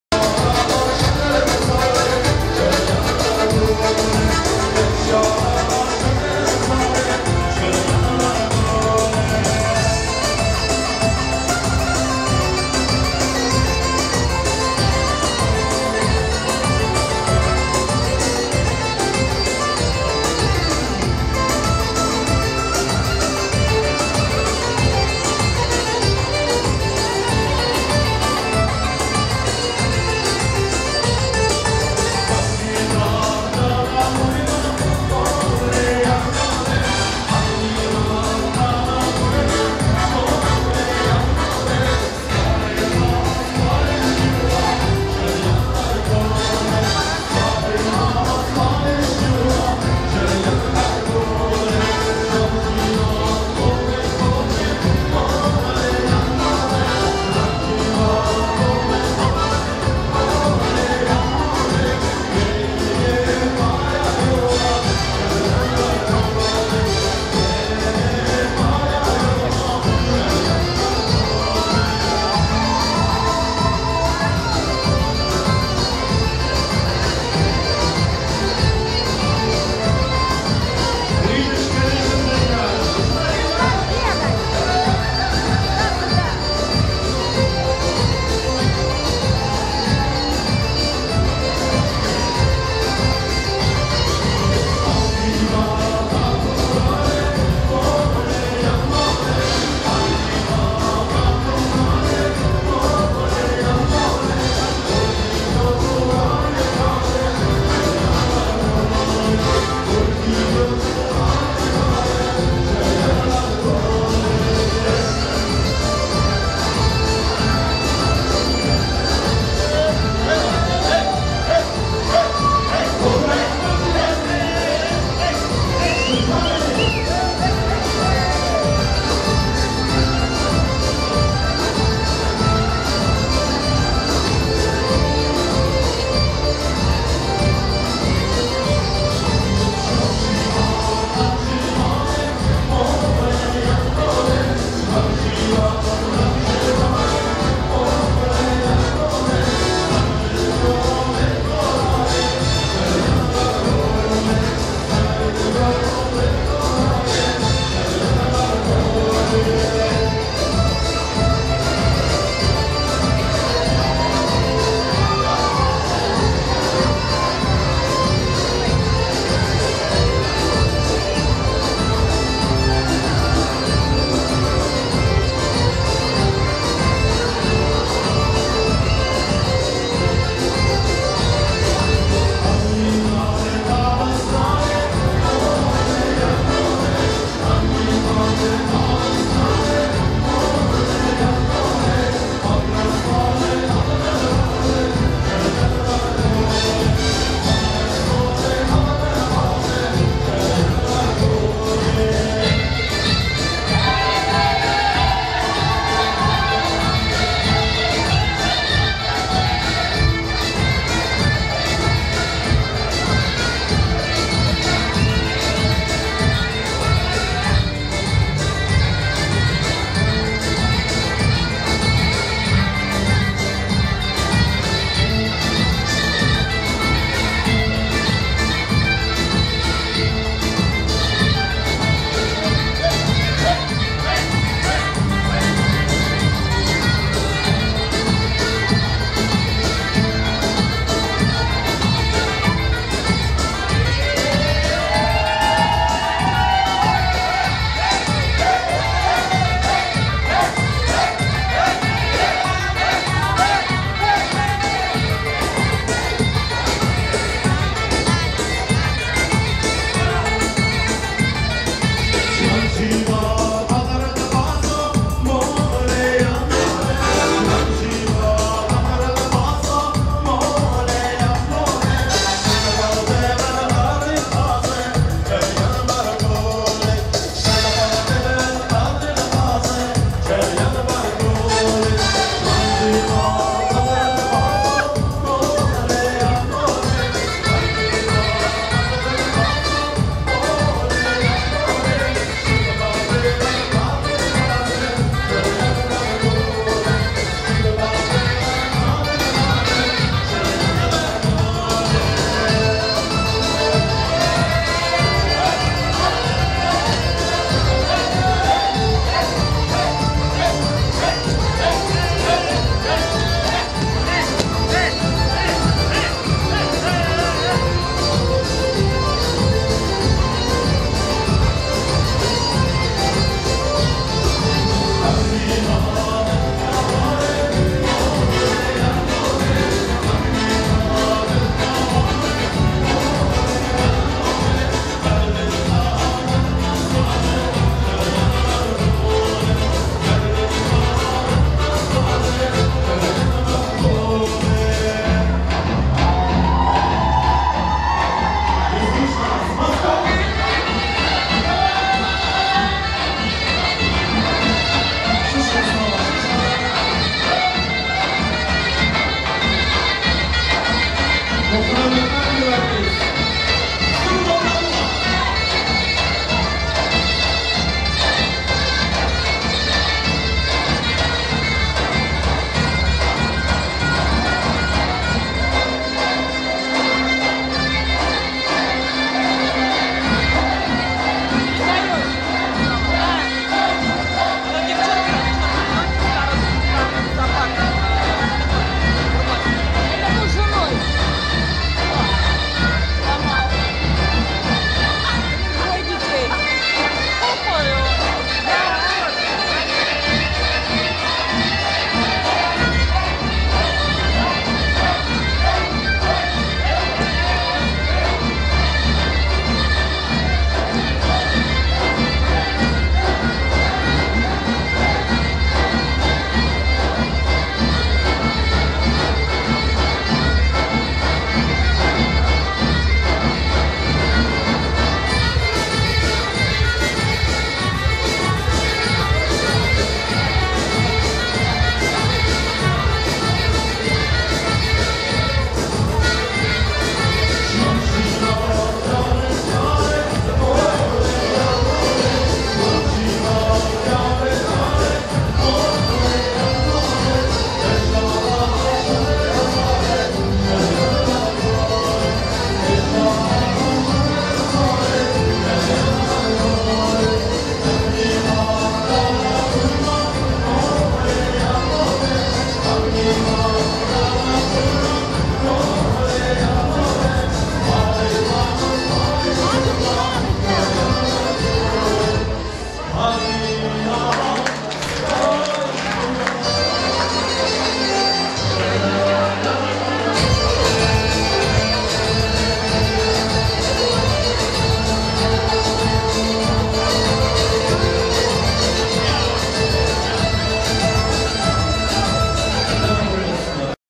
Ассирийцы.Духрана д Мар Гиваргис. 🎊🙌🙏Assyrians .Dukhrana d Mar Givargis.🎊🙌🙏16/11/2014
День памяти Святого Георгия ,ноябрь.2014